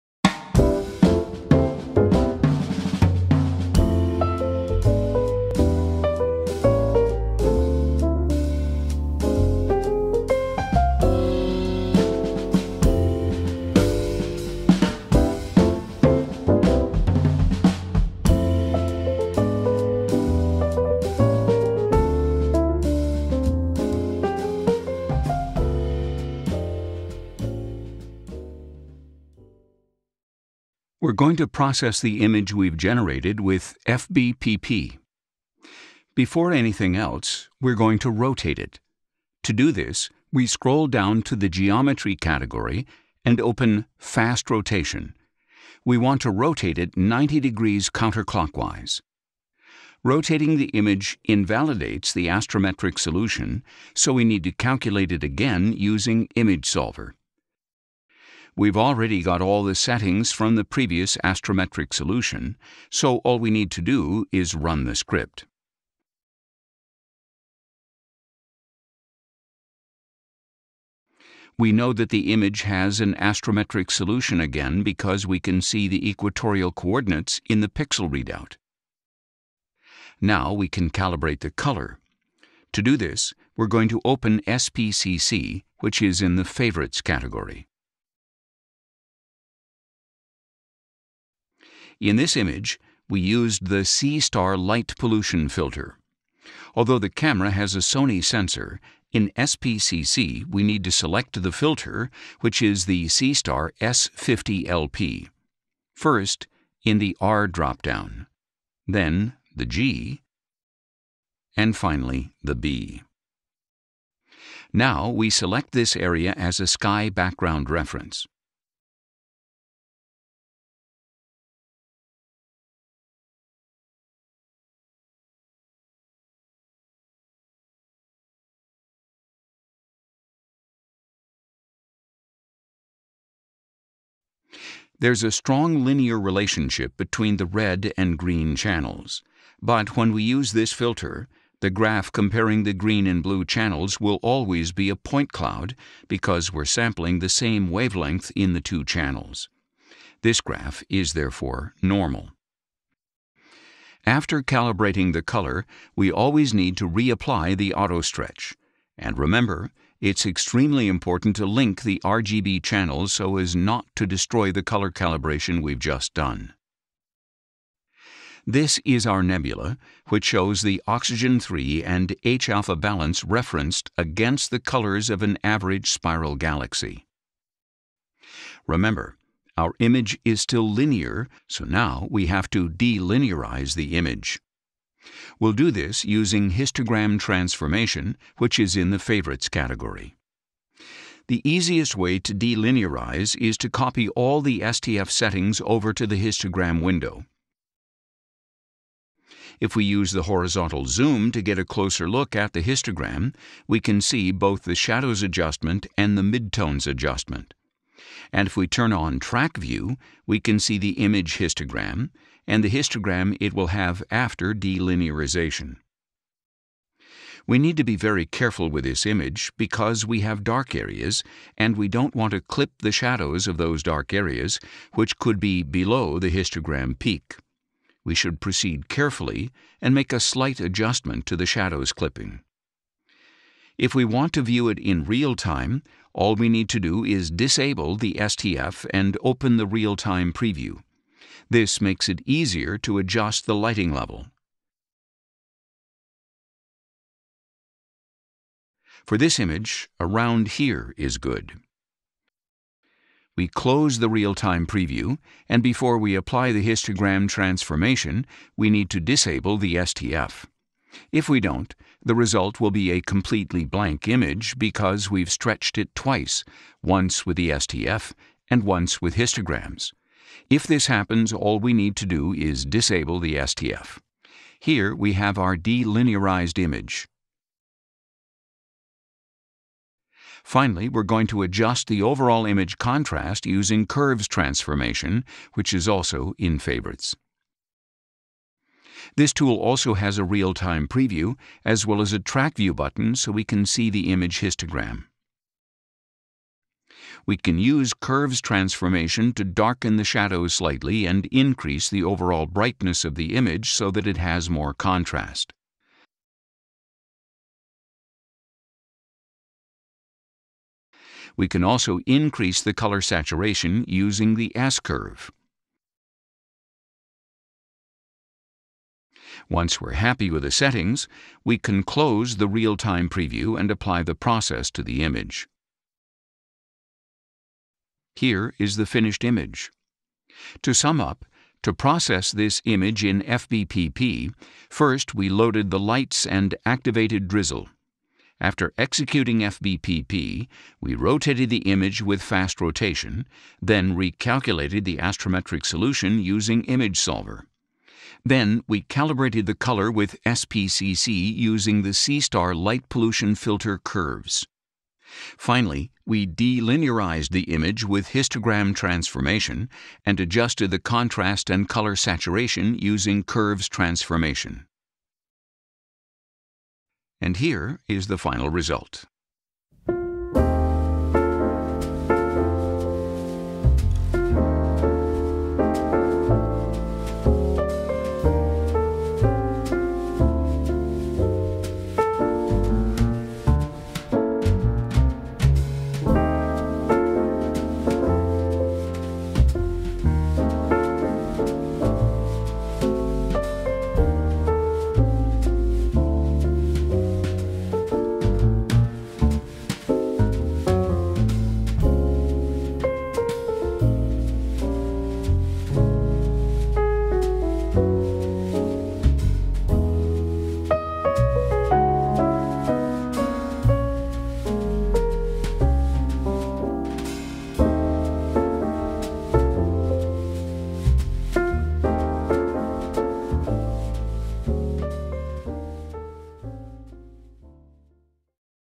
NGC 7000 with a Seestar S50 (II)
A complete preprocessing and postprocessing example using data acquired with a Seestar S50 smart telescope (Part 2).

00:30 Image rotation and plate solving
01:19 Color calibration
02:57 Delinearization
04:40 Contrast adjustment
05:30 Processing summary